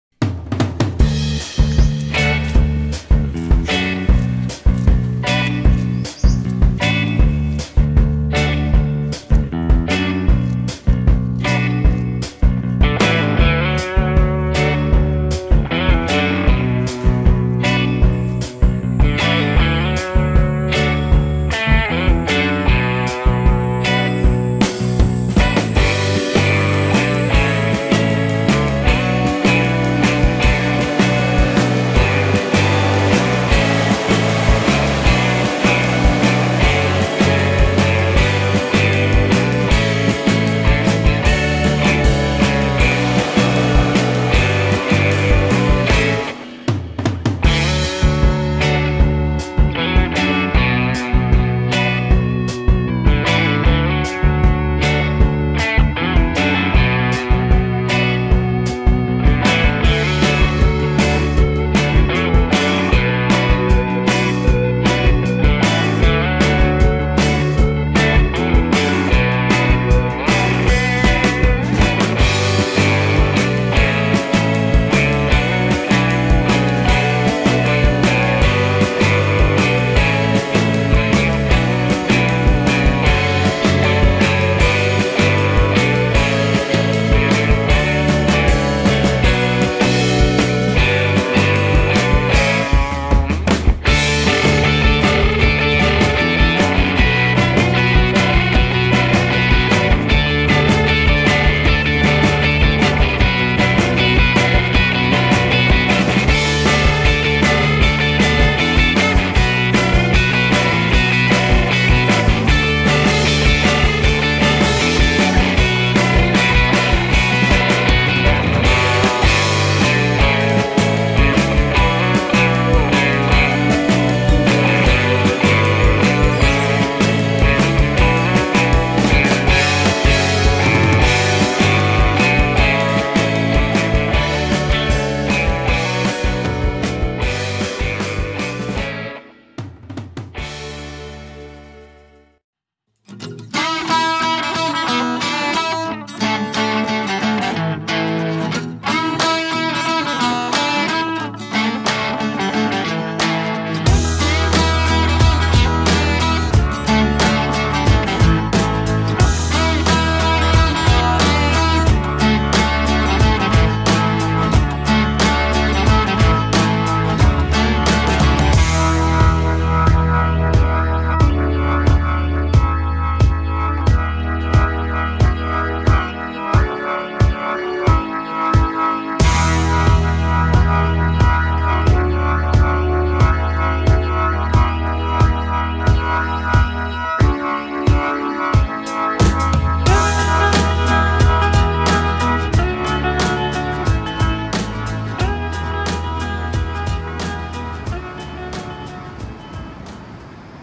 Nicaragua fauna - monkeys, crocodiles, insects, birds, frogs etc.
keep your eyes open and discover Nicaragua's rich fauna: howling monkey, crocodile, leafcutter ants, iguana, frogs, vultures, butterflies, ravens, locust, crabs, turtle,  horses, pigs, cow, etc.